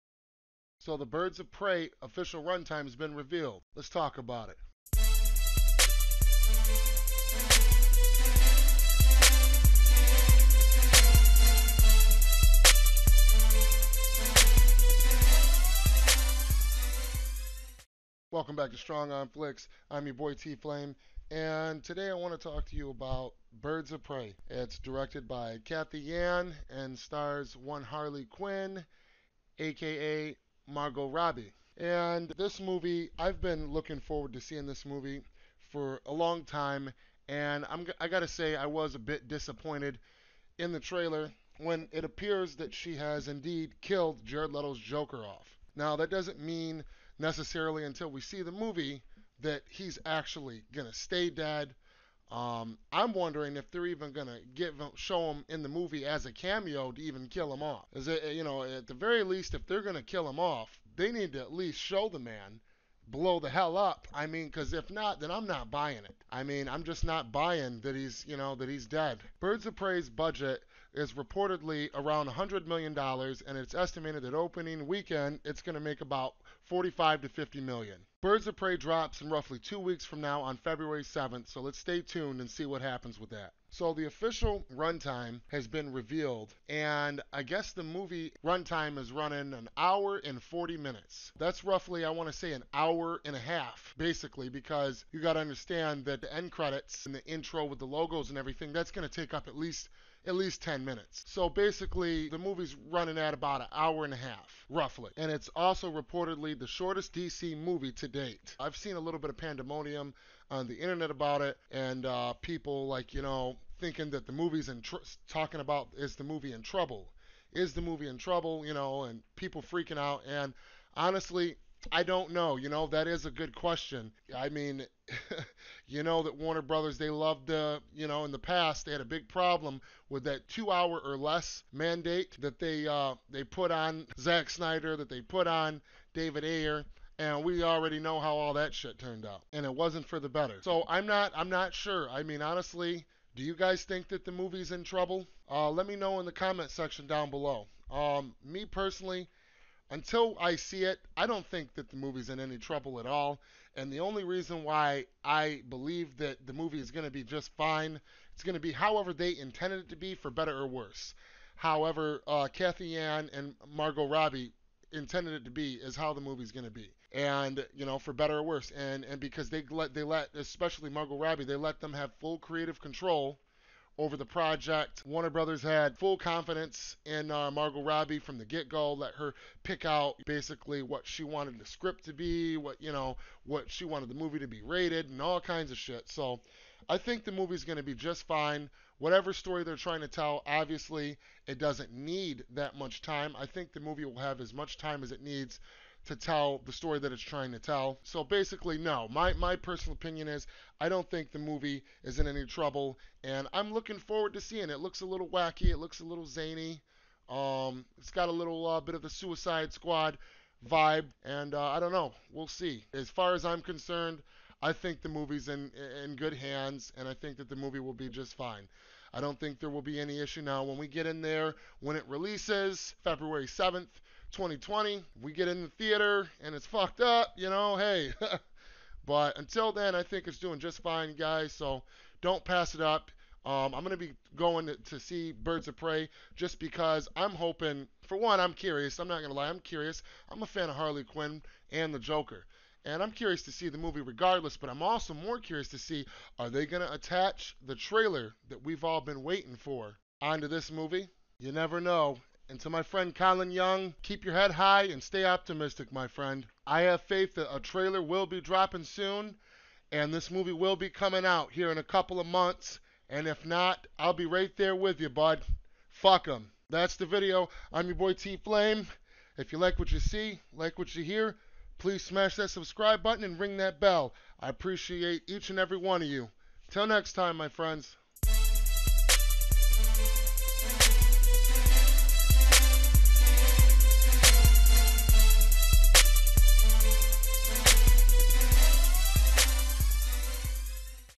Birds of Prey Runtime spells trouble? Possible Snydercut trailer?
Welcome to Strong Arm Flix! I'm ya boy Tflame Gladiator!

Strong Arm Flix Episode 18

This is a small channel so if you like what you